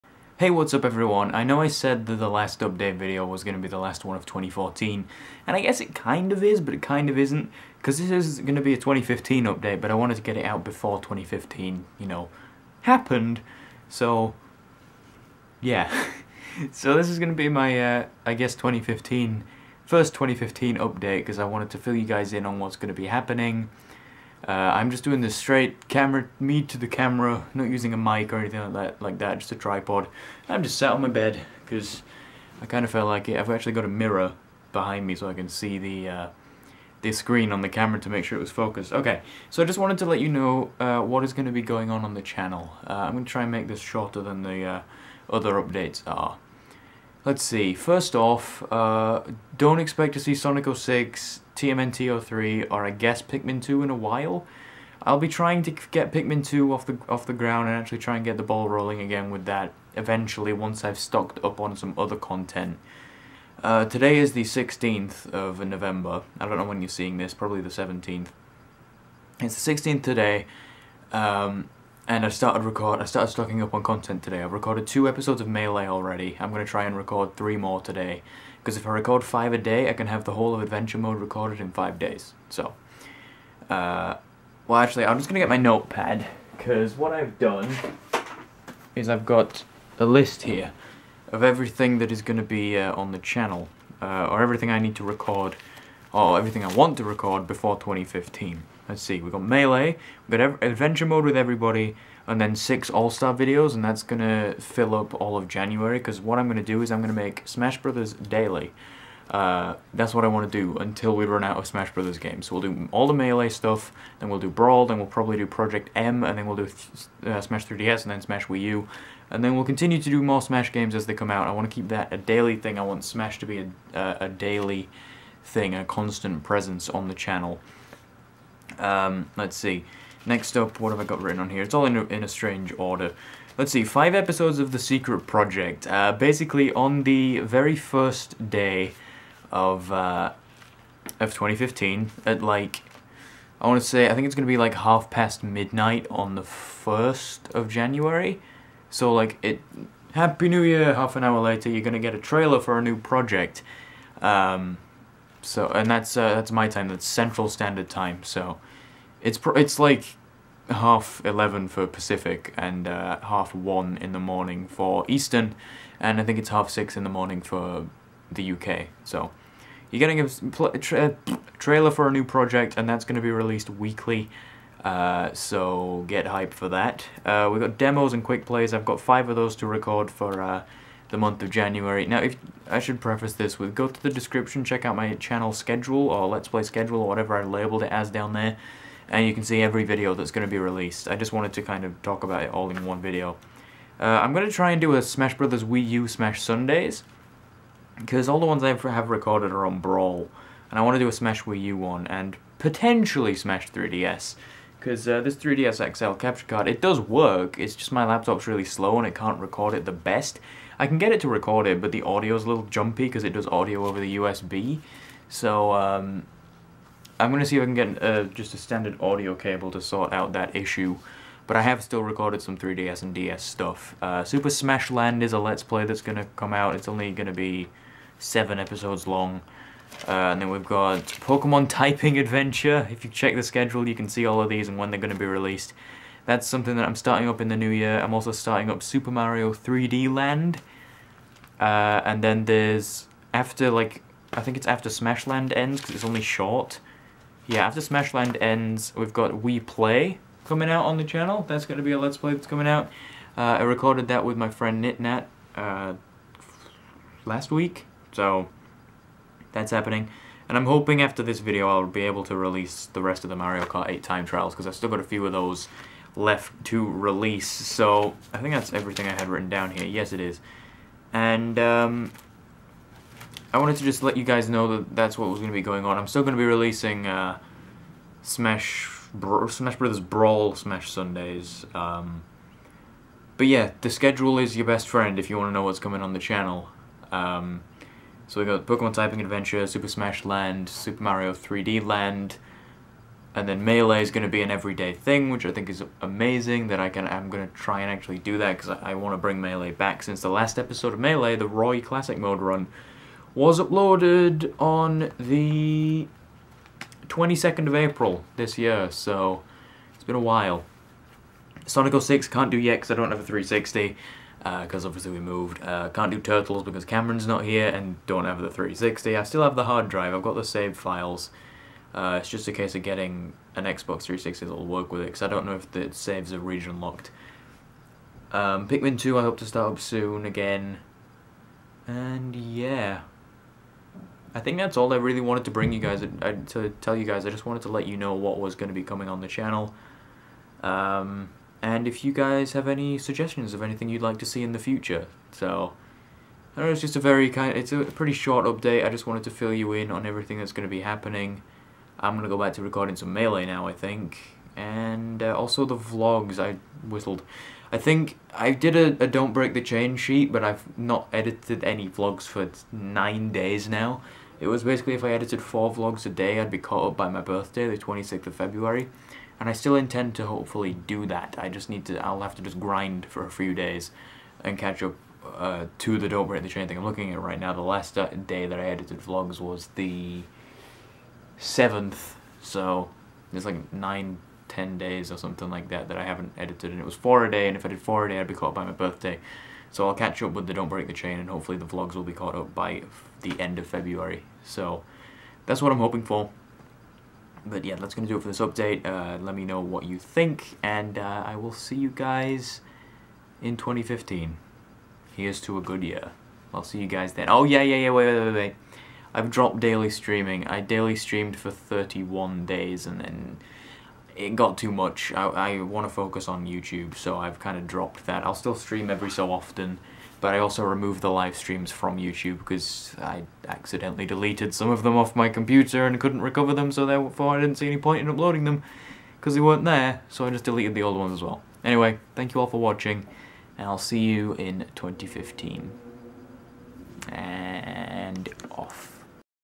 Here's to 2015
Me talking to you about what's to come in 2015. I'm hoping it will be an awesome year!

Would you like to suggest a future Let's Play or video series? Leave a suggestion in the comments below and I might consider it!